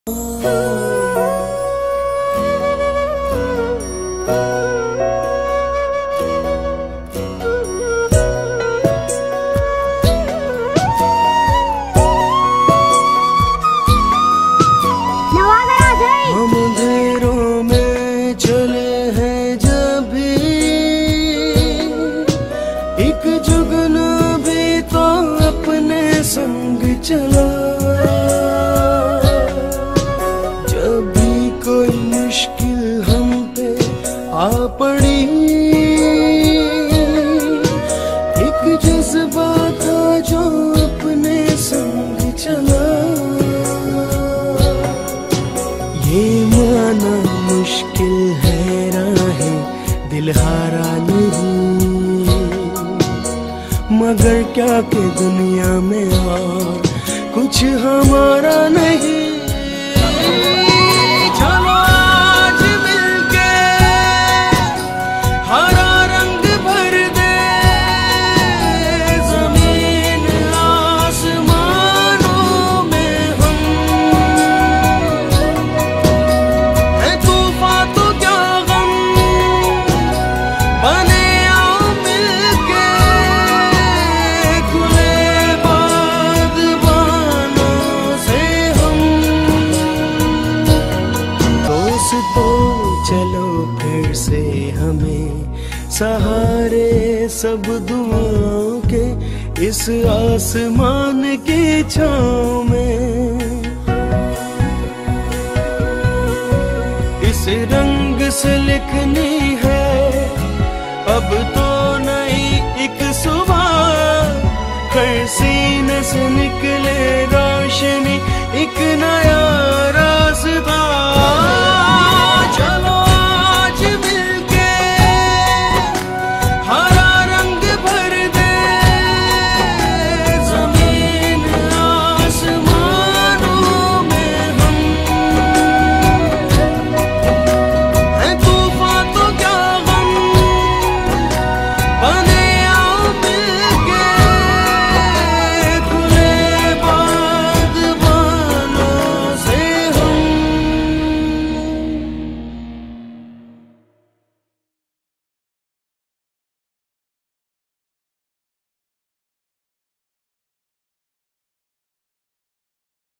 Hara Rang Bhar dyn || THE NATIONAL FLAG OF PAKISTAN
Around the world, no one can see such mockery of the national flag, whereas the practice is being followed for the last few years and no one has taken any notice. This distortion of our graceful flag is an irresponsible attitude and affects the dignity of our beloved flag. Our National flag should only fly high